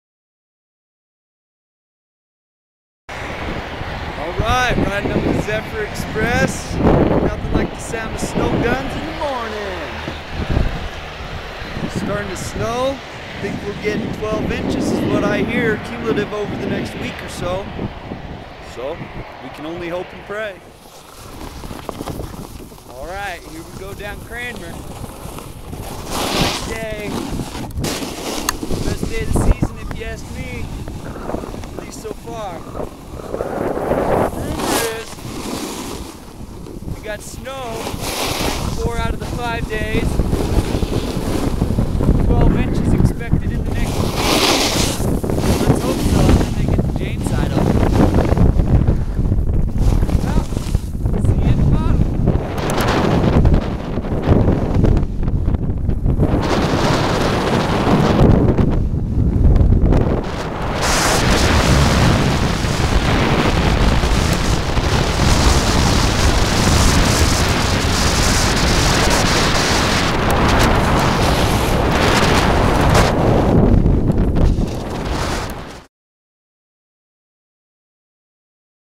Friday, Dec 7 Sky Hi News Mountain Report 2
Check out the conditions at Winter Park Resort on Dec. 7, 2012